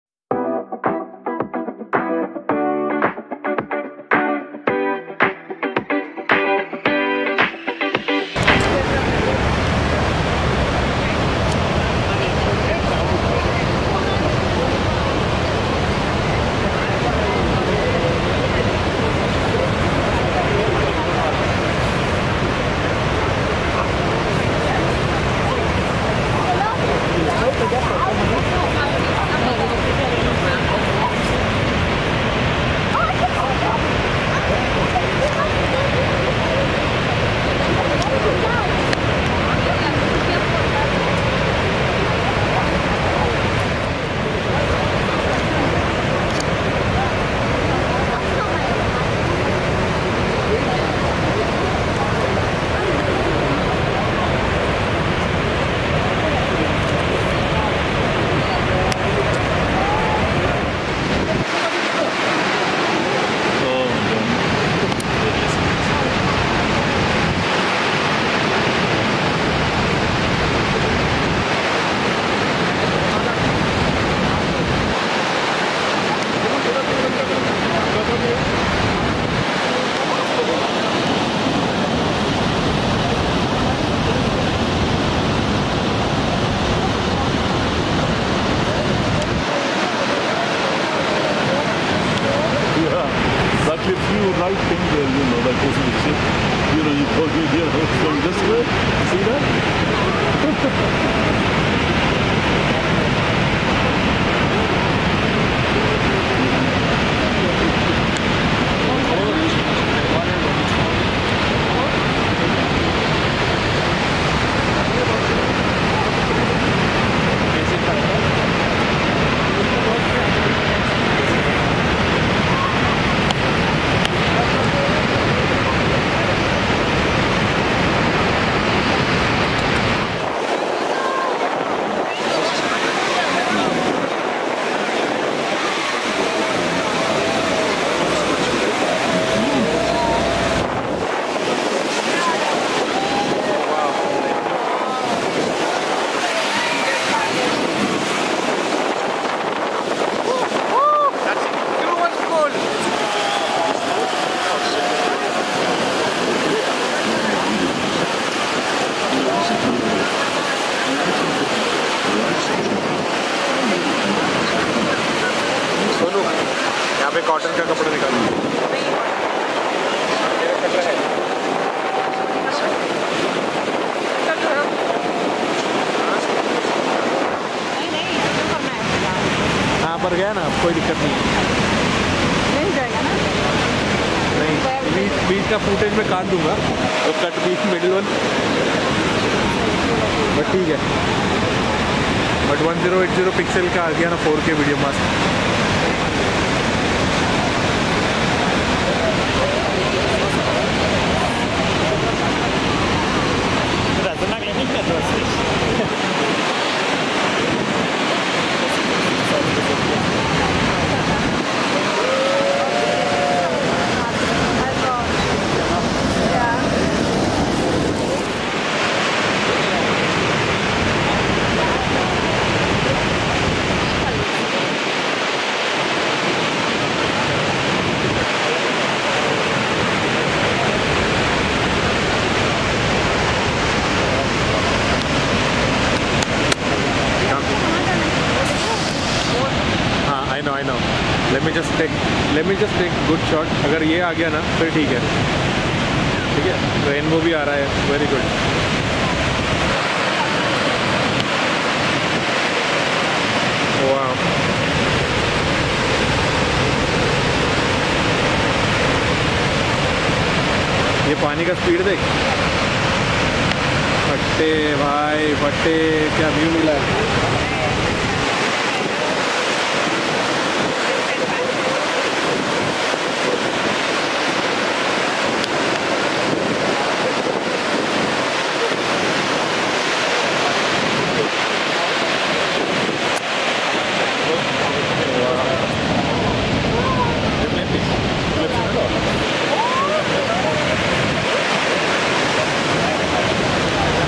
Niagara Falls 2024
This video recording had multiple obstacles while recording and have combined for your ease. This was shot in afternoon for better visibility purpose and due to summer rush, camera was not steady, and I am sorry for the mess. I also tried to keep the public talk away, but I had to record the water fall noise intact so could not filter it.